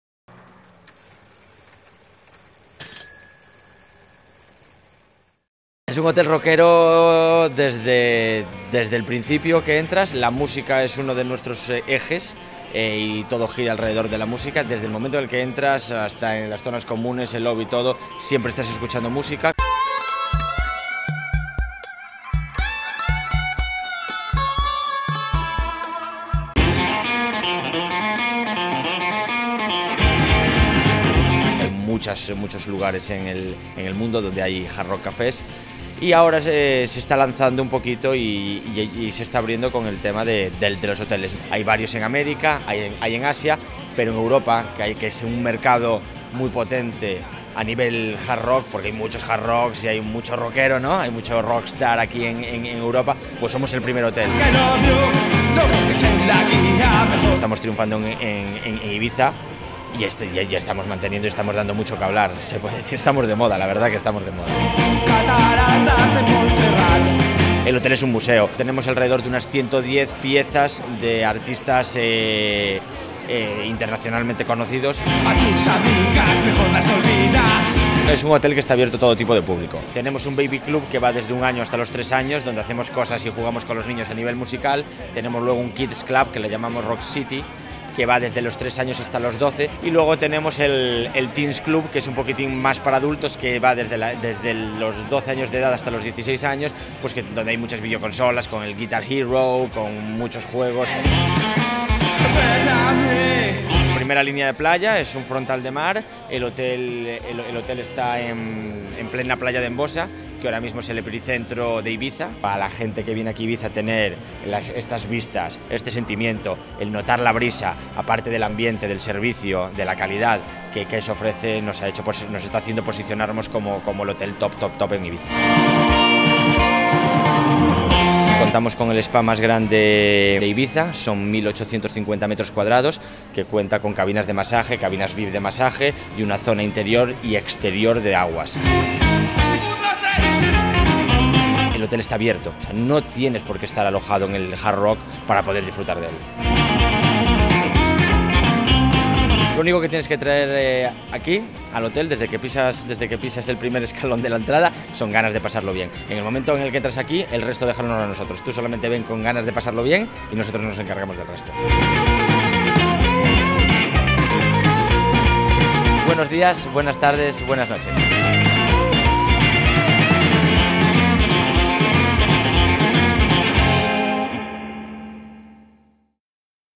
Hard Rock Hotel Ibiza - Tendencias.tv #687
Recién estrenado, el Hard Rock Hotel Ibiza se inauguró el pasado 13 de Junio y no quisimos perdernos la oportunidad de disfrutar de sus instalaciones. Allí comprobamos por qué es el lugar escogido por tantas celebrities y personajes famosetes que visitan la isla. Siendo el primer hotel Hard Rock de Europa, no nos sorprendió el buen ambiente que se respiraba en todas las áreas del recinto: la zona de juegos para pequeños y no tan pequeños, las dos piscinas en las que relajarse, el magnífico spa, las diferentes opciones de restaurantes... mil rincones en los que perderse y disfrutar. No olvidéis que la protagonista del Hard Rock es la música y que cuenta con una colección de objetos increíble y en el Hard Rock Hotel Ibiza pudimos ver una selección: Un traje de John Lennon o de Elvis Presley, una bata de Michael Jackson, un vestido de Miley Cirus, Rihanna o Shakira. Pasamos un día redondo, dadle al play para comprobarlo.